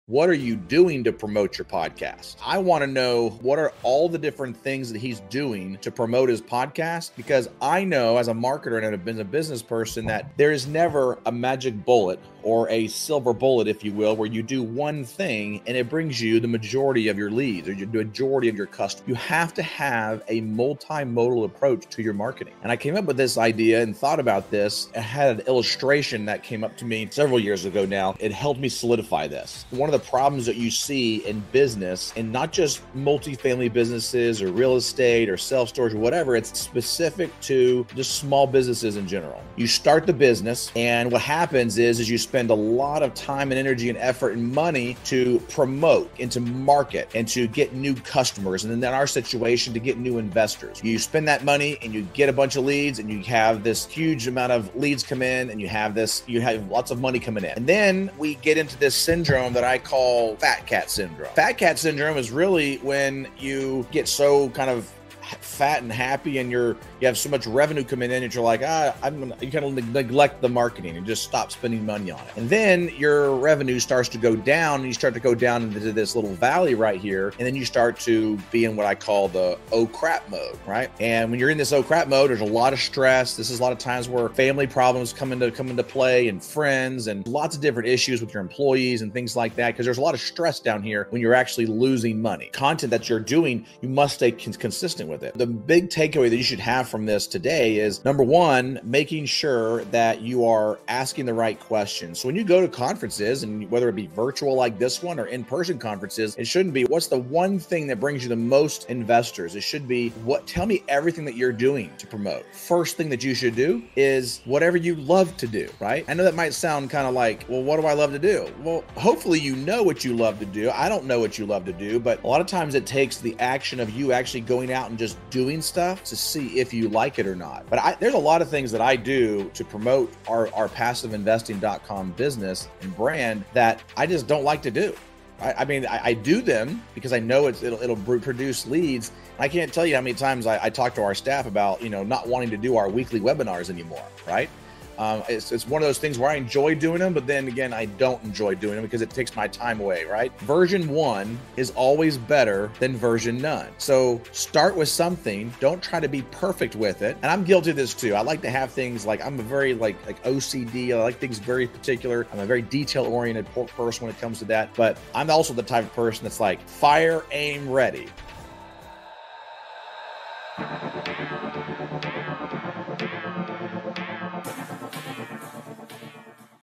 Building A Marketing A Machine To Raise Massive Capital Dan Handford
Check out the highlights from Dan Handford's talk at the Fall Mid Atlantic Multifamily Investing Conference.